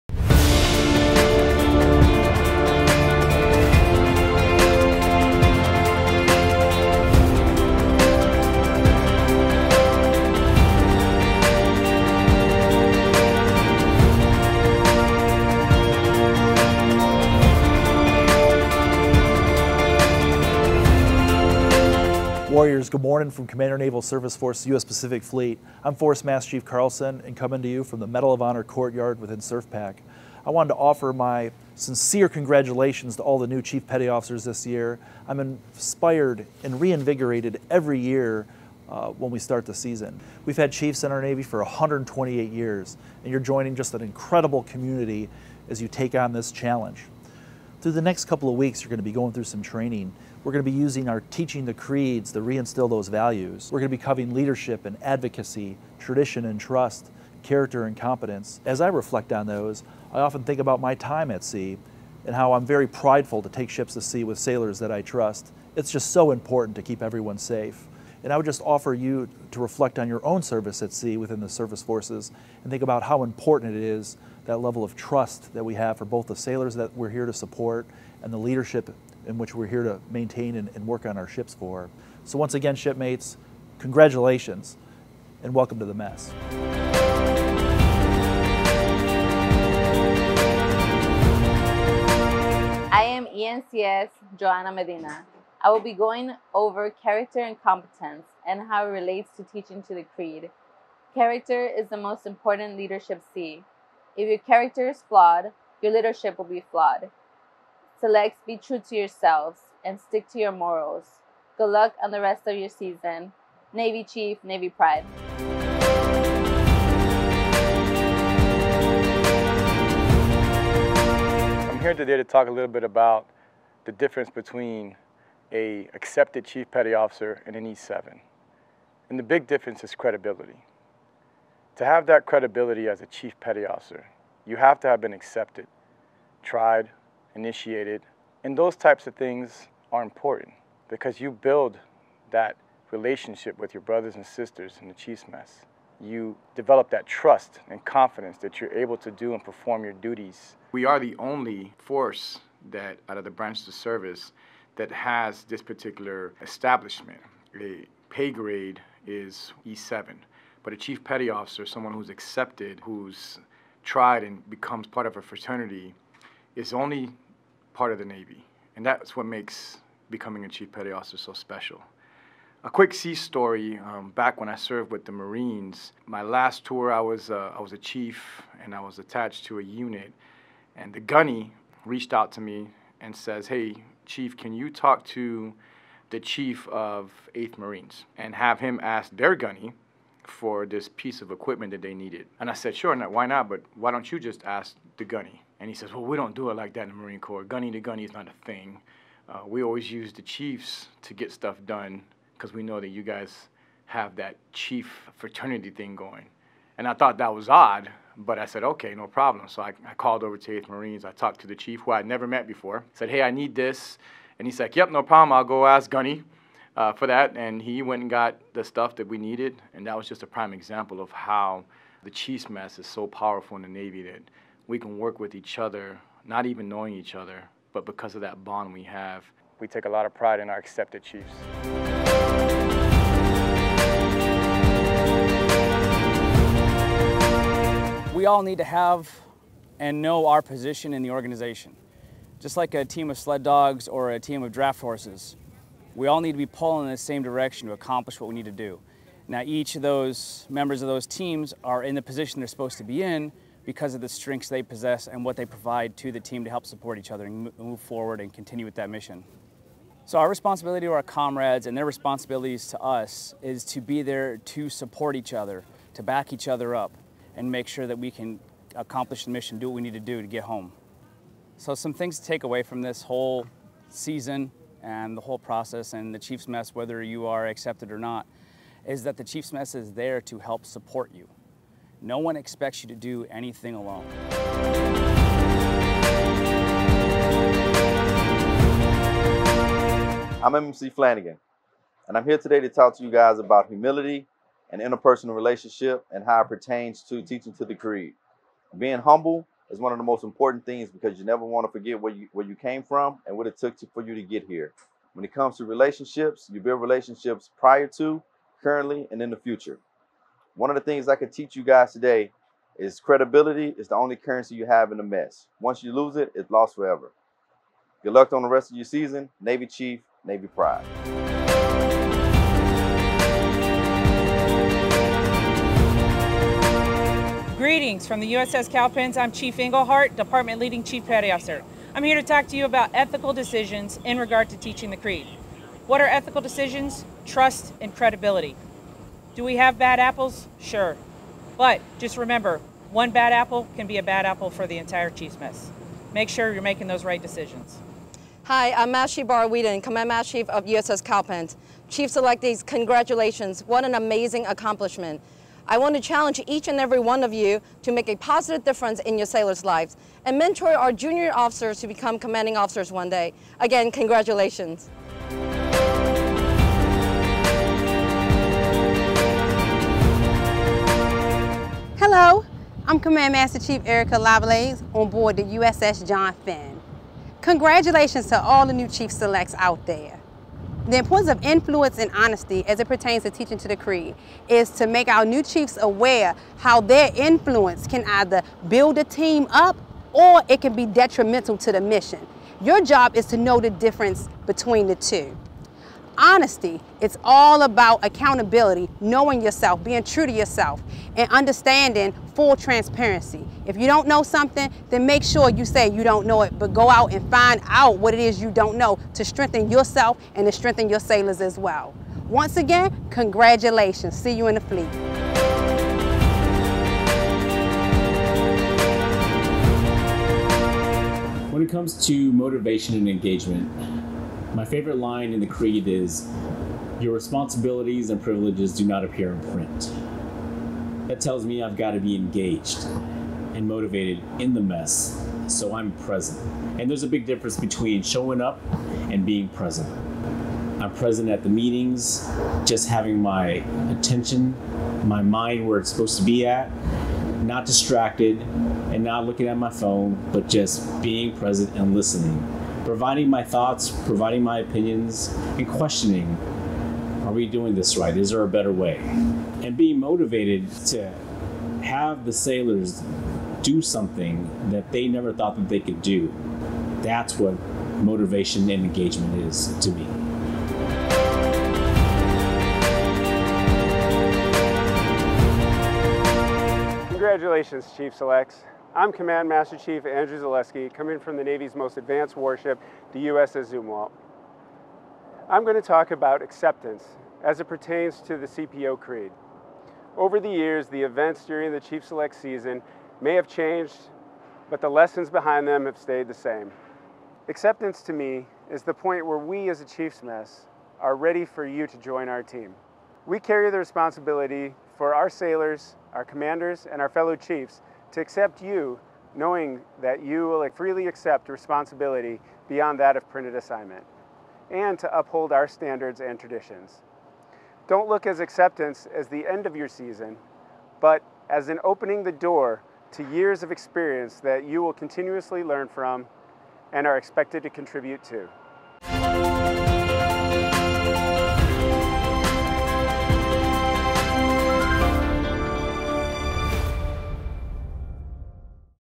Teaching to the Creed Compilation
Hey SurfaceWarriors its Chief initiation season! This year we went to the waterfront and gathered knowledge and advice from senior members of our Chiefs mess for a nine part video series, "Teaching to the Creed."

ICYMI, the nine parts consist of; leadership, advocacy, tradition and trust, character and competence, credibility of a chief, teamwork and loyalty, humility, interpersonal relationships, ethical decisions, influence and honesty, motivation and engagement, and acceptance.
This is Following Office of the Master Chief Petty Officer of the Navy’s 2021 Chief's initiation guidance.

"Each of our selectees must understand what is expected of them as a Chief Petty Officer, and leveraging our Creed as the primary focus will accomplish this goal," said Master Chief Petty Officer of the Navy Russell Smith.